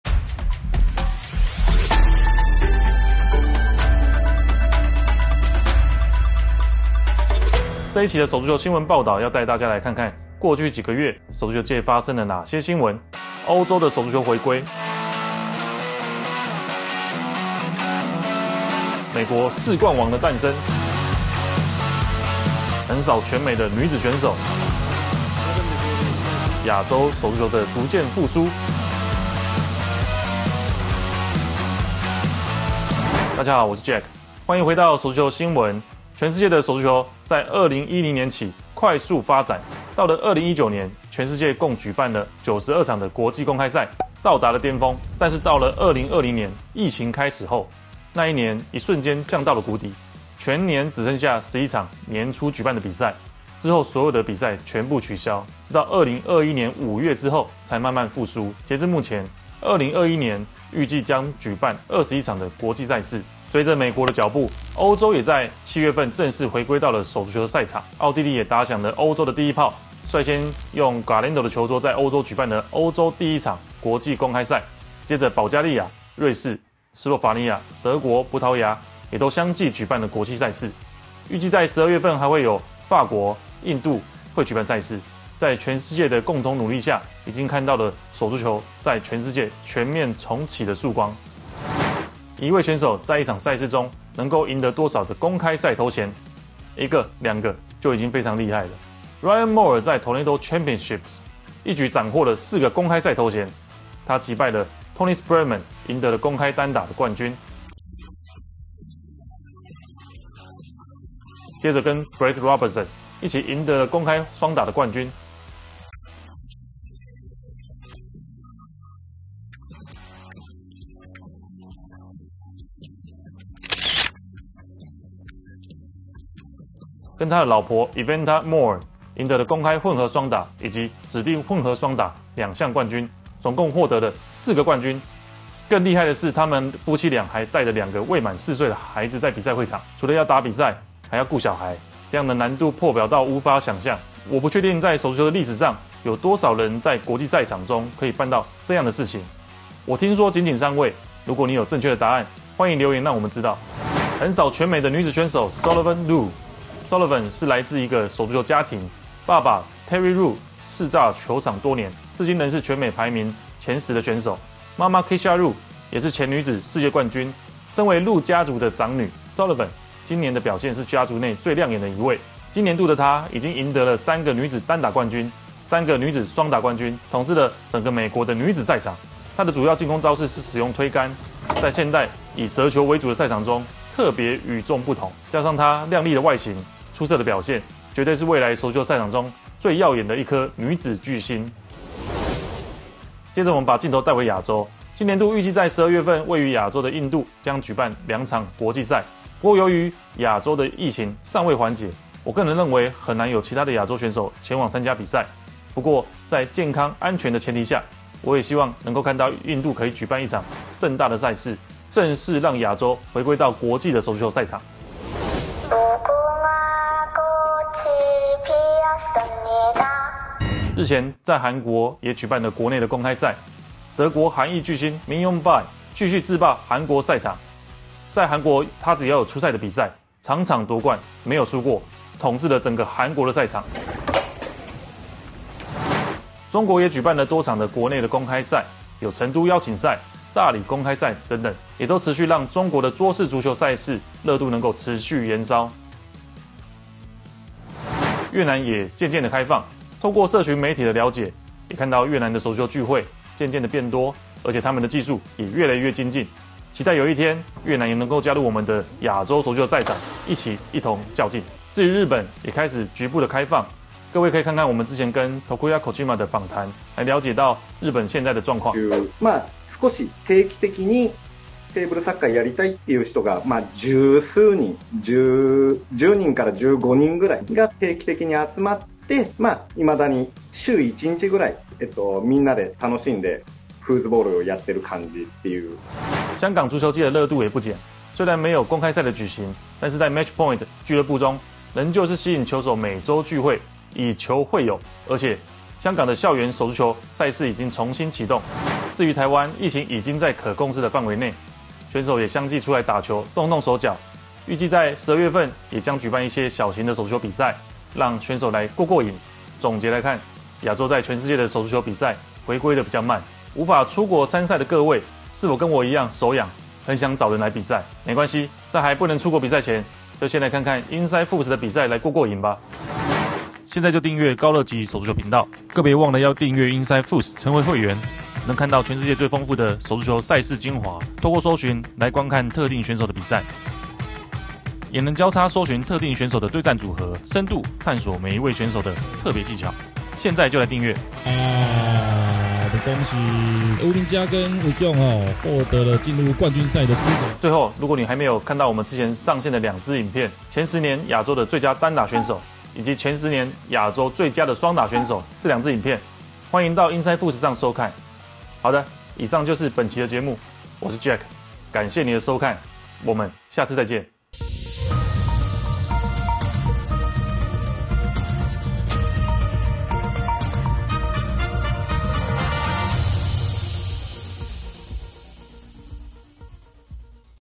Foosball News with Jack Huang | Dec 2021
In this episode of foosball news update

00:00 -Intro
01:03 -Europe is back
01:31 -Quadruple Crown
02:33 -New Foosball Queen
03:12 -Asia foosball is coming back